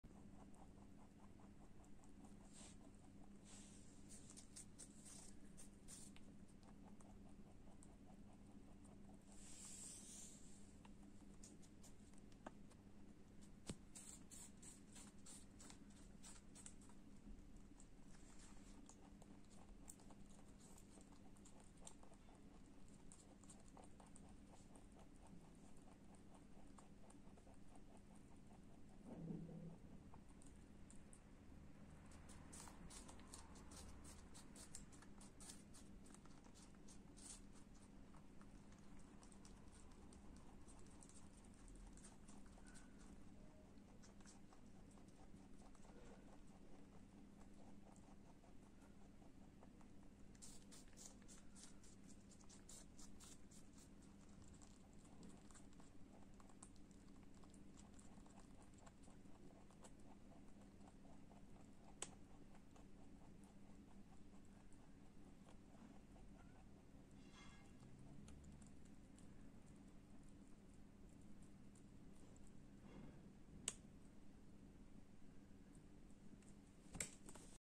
Apples are bunnies favor 🤤 sounds on🔊
listen carefully, it's kopol's chewing time 😎